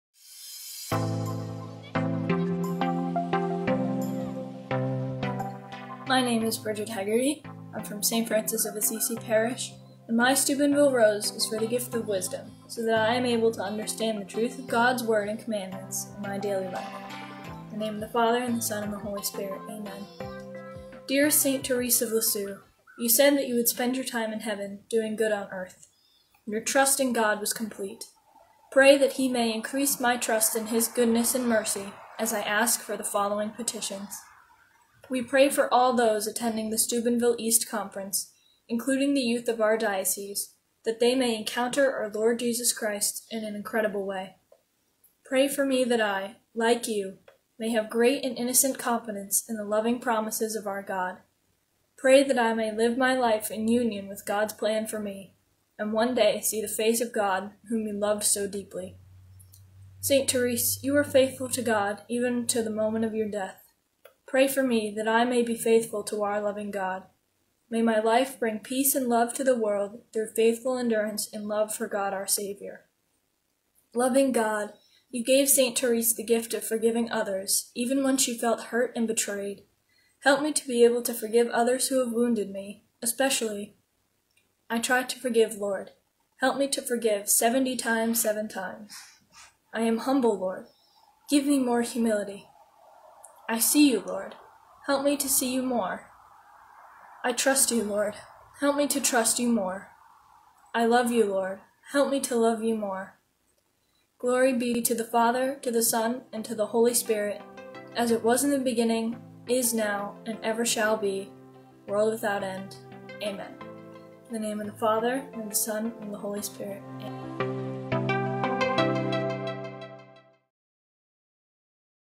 Saint Therese of Lisieux Novena Day 5
In Preparation for Steubenville East 2022 Day 5 of a Novena to St. Therese of Lisieux.